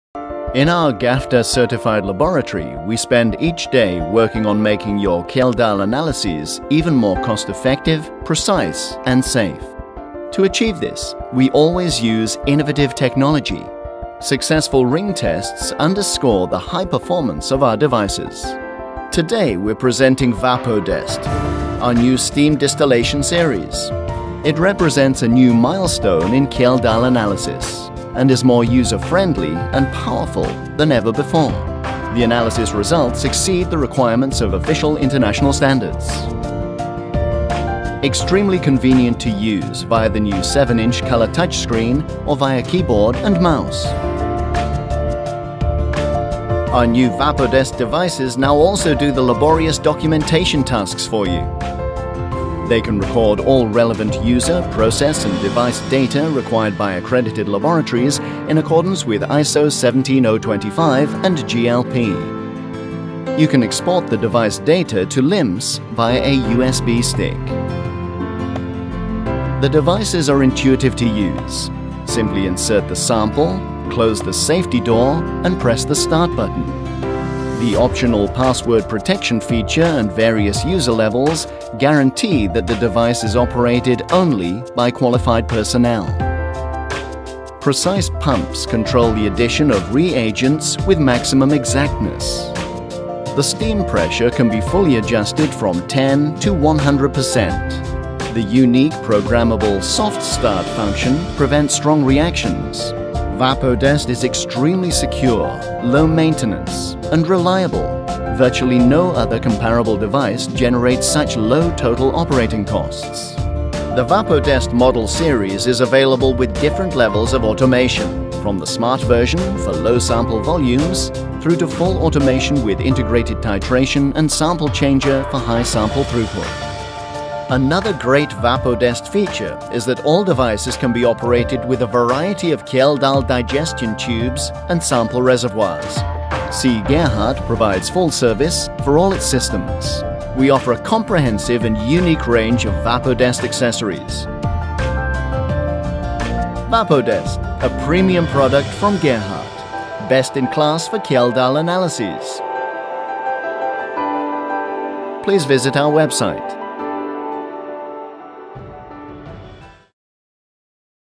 VAPODEST - La soluzione ottimale per il metodo Kjeldahl
VAPODEST è attualmente il sistema di analisi più moderno ed efficiente disponibile sul mercato per la determinazione di azoto basata sul metodo Kjeldahl